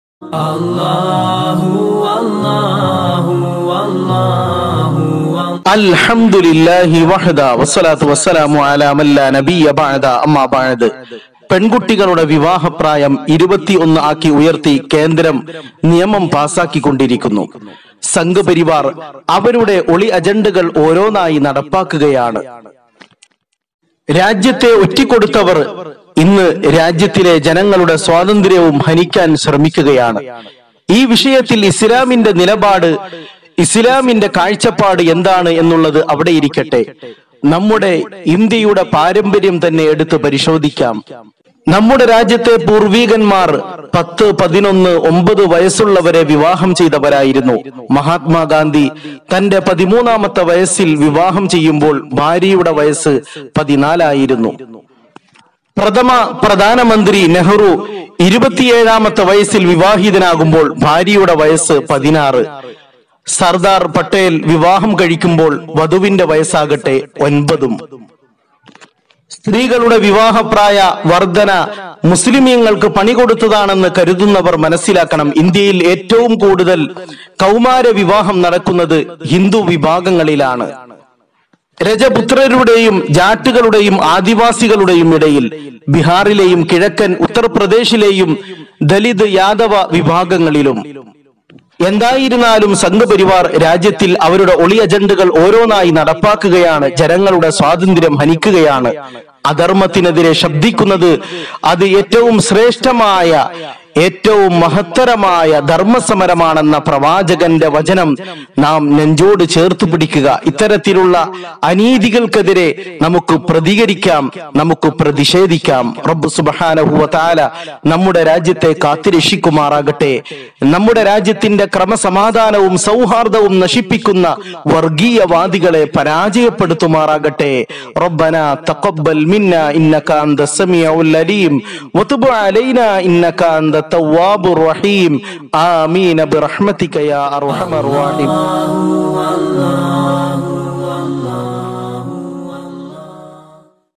വിവാഹം 21ലേക്ക് ഗാന്ധിയും നെഹ്രുവും പട്ടേലും ചെയ്തത് തിരിച്ചടിയാവുന്നു
വിവാഹ പ്രായം  21ലേക്ക് ഗാന്ധിയും നെഹ്രുവും പട്ടേലും ചെയ്തത് തിരിച്ചടിയാവുന്നു

Topic :-